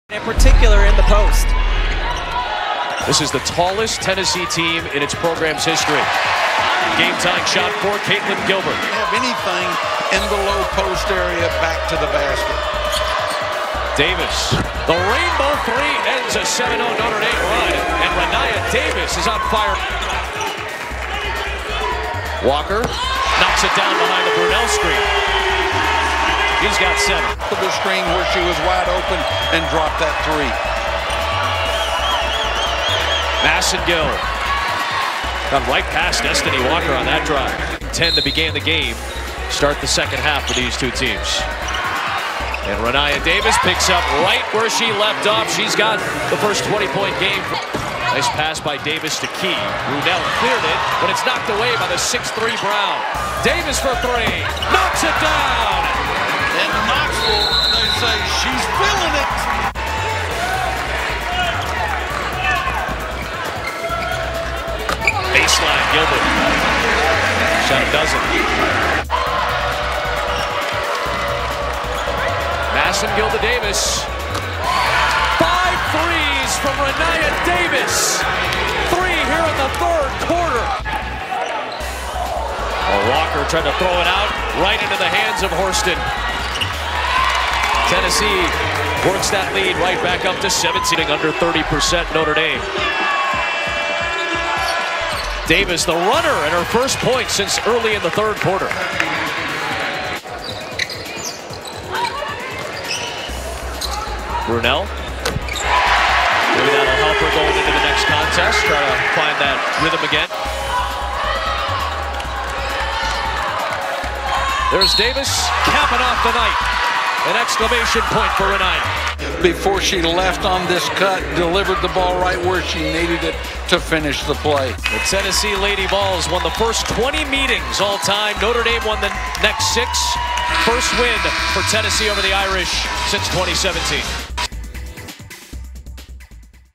Tennessee vs. Notre Dame Women's Basketball Highlights (2019-20)
Tennessee vs. Notre Dame: The Notre Dame Fighting Irish came up short in a tough game against Tennessee, 74-63.  Destinee Walker led Notre Dame with 18 points in the loss.  The shots just weren't falling for Notre Dame, as they shot just 31% from the field.  Rennia Davis scored 33 points for the Lady Vols in the win.